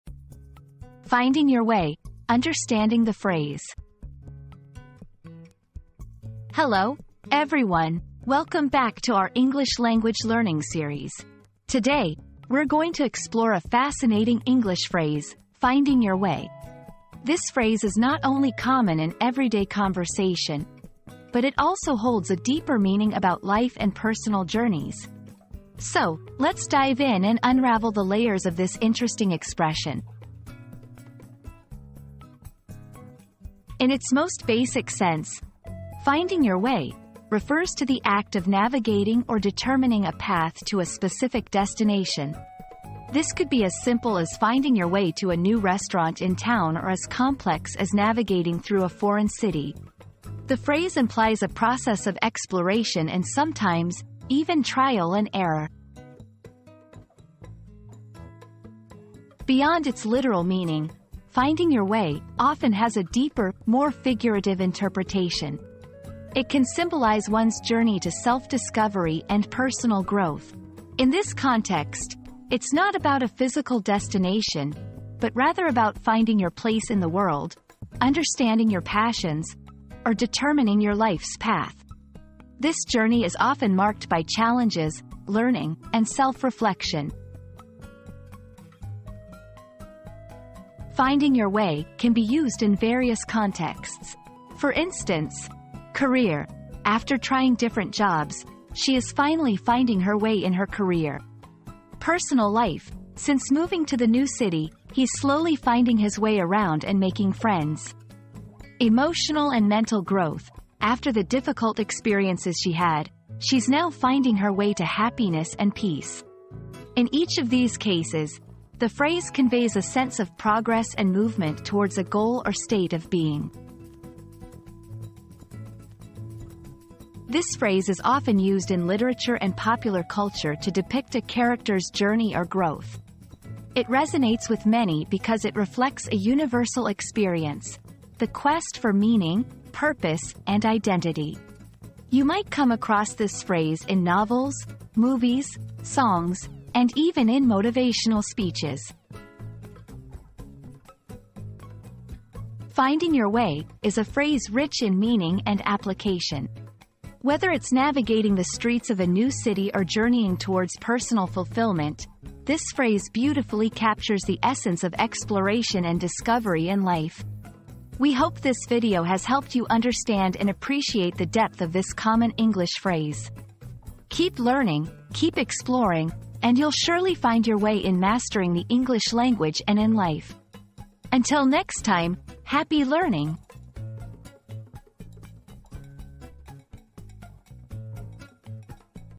Finding Your Way: Understanding the Phrase
Finding Your Way: Unlocking the Meaning • Discover the true essence of the phrase 'Finding Your Way' and gain valuable insights on navigating life's challenges with confidence and purpose. Join us on this enlightening journey of self-discovery and empowerment.

00:00 • Introduction - Finding Your Way: Understanding the Phrase
00:33 • Literal Meaning of "Finding Your Way"
00:58 • Figurative Meaning of the Phrase
01:30 • Usage in Everyday Language
02:06 • Cultural and Literary References